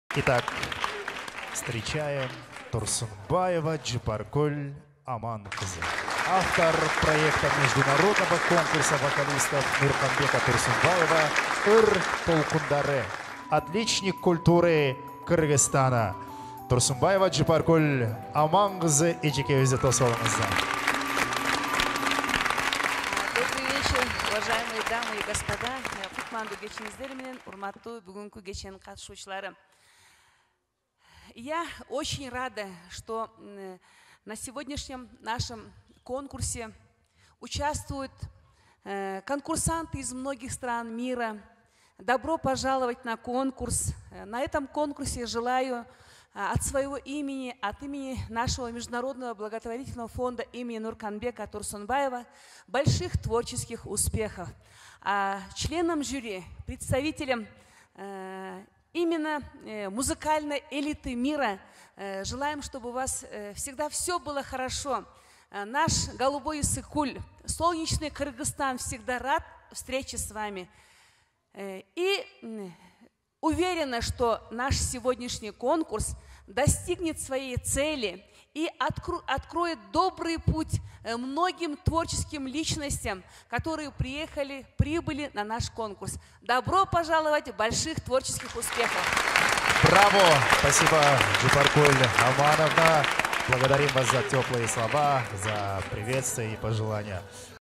Международный конкурс вокалистов Нурканбека Турсунбаева "ЫР ТОЛКУНДАРЫ - 2018"
Международный конкурс вокалистов Нурканбека Турсунбаева "ЫР ТОЛКУНДАРЫ - 2018", посвященный в честь 75 летнего юбилея легендарной актрисы Таттыбубу Турсунбаевой (открытие конкурса). Выступление автора проекта, Отличника Культуры Кыргызской Республики, председателя жюри  Турсунбаевой Джыпаркул Аман кызы.